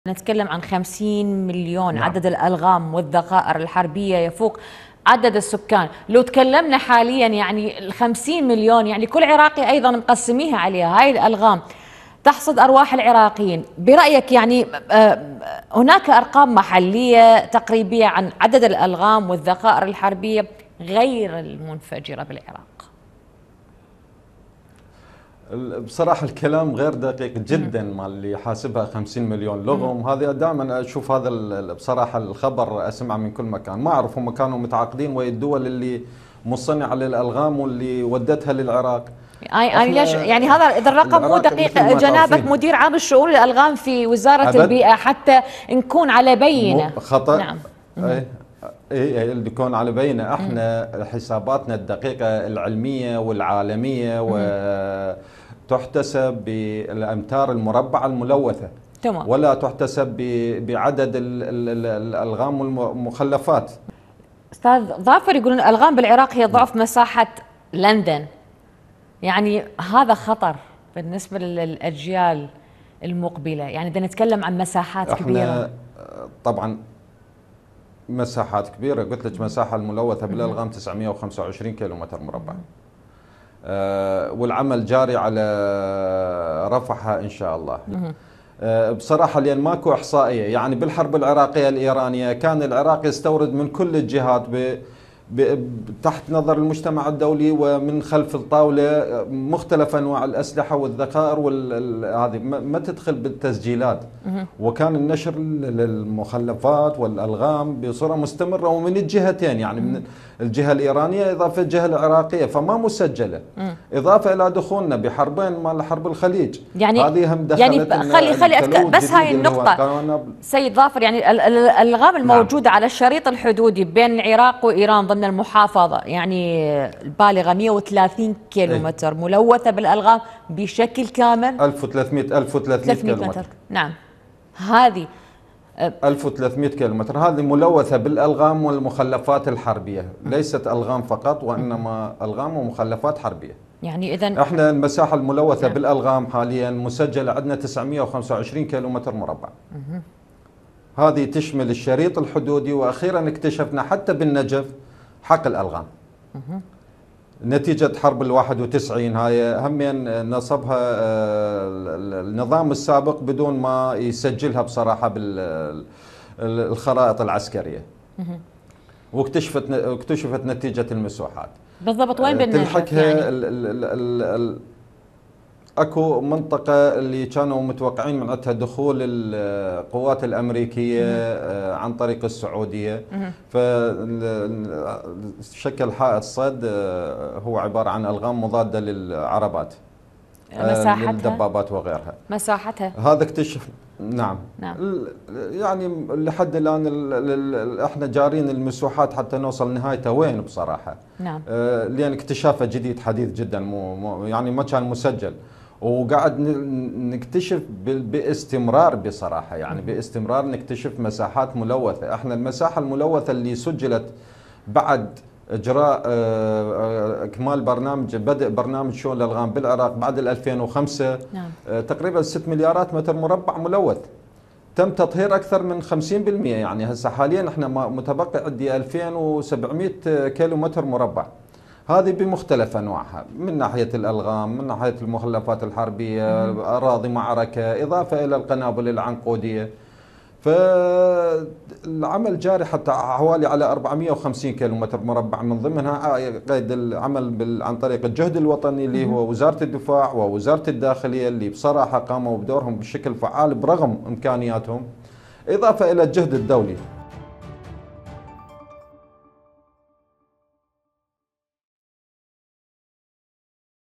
لغم لكل عراقي .. ملايين الألغام تحصد ارواح المواطنين
موقع قناة التغيير الفضائية الالكتروني